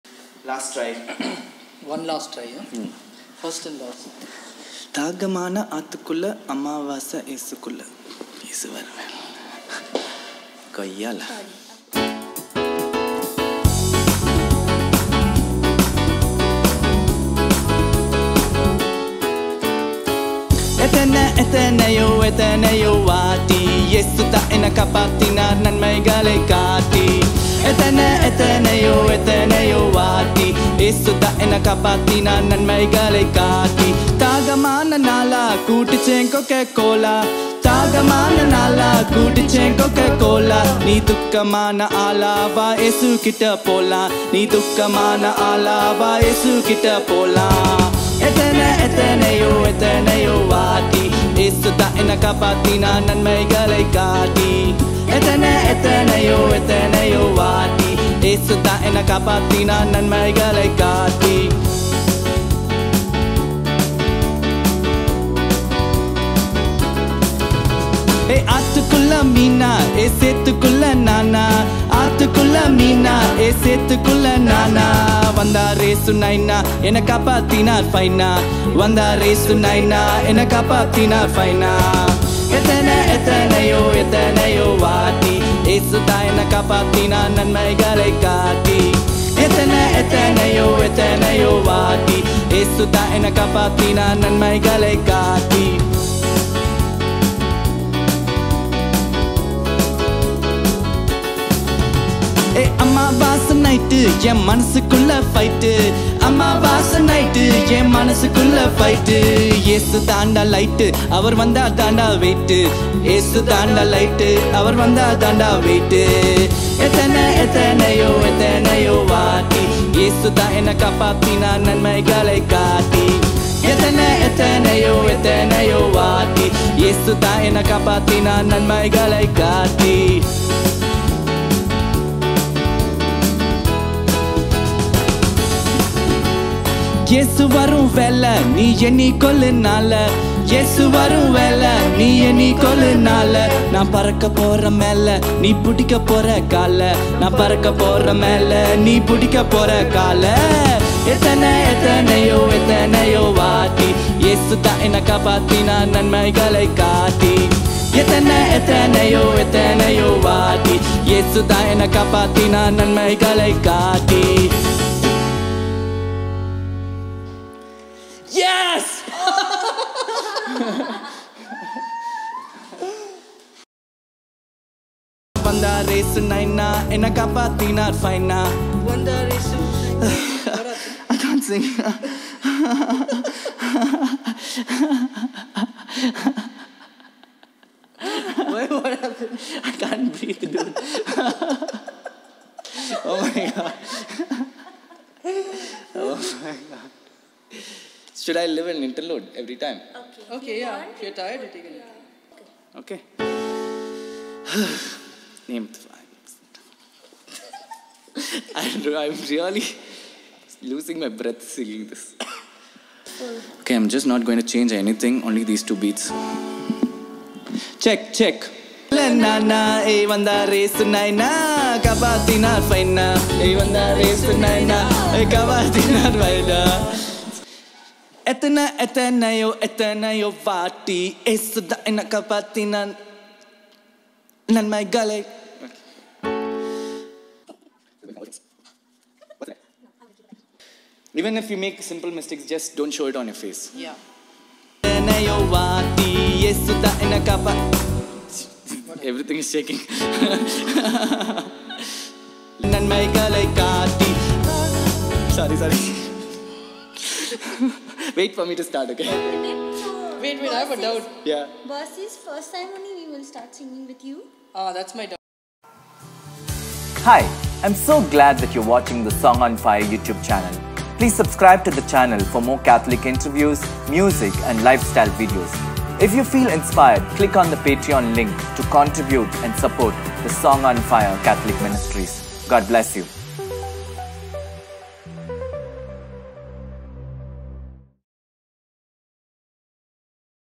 TAMIL Christian ACTION Song || Joseph Dinesh (JD ) || Song on Fire ( Music )
I thank my good friend for singing this Christian song for the Song on Fire ( Music ) Channel - Let this song be a blessing to you - God Bless

For Catholic Interviews, Commentaries, Movie reviews, Testimonies, and Debates